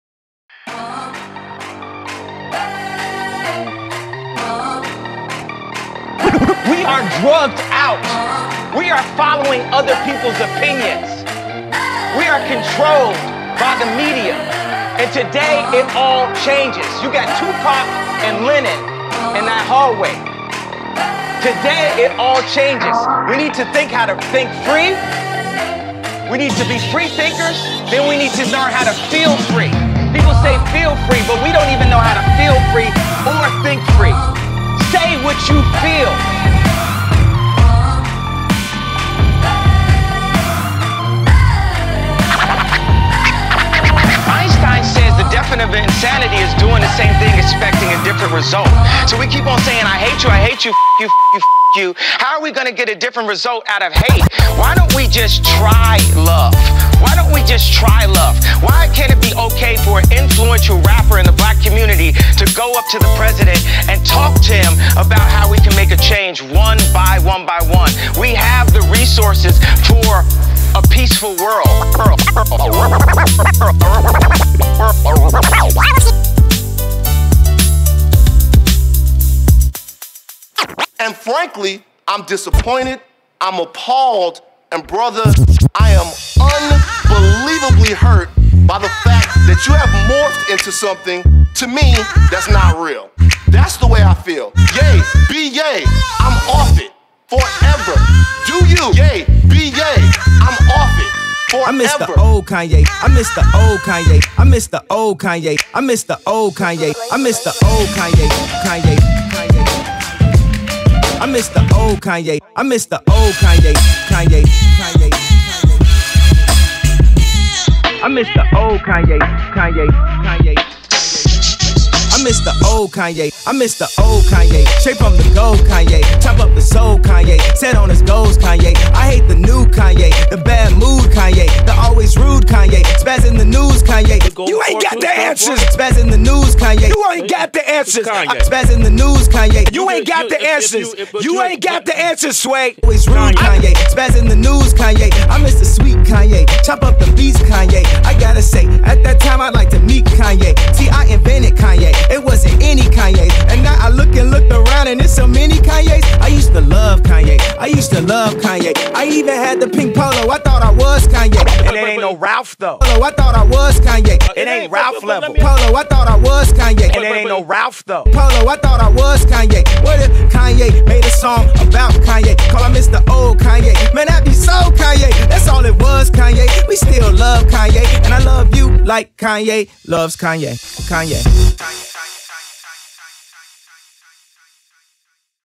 Kanye West, TMZ 2018 INTERVIEW MIX
Just made a quick mix (sort of with Yay's recent interview with TMZ. right or wrong, I admire this dude and wanted to make a quick mix up including some quotes yay brought up with some beat in the background.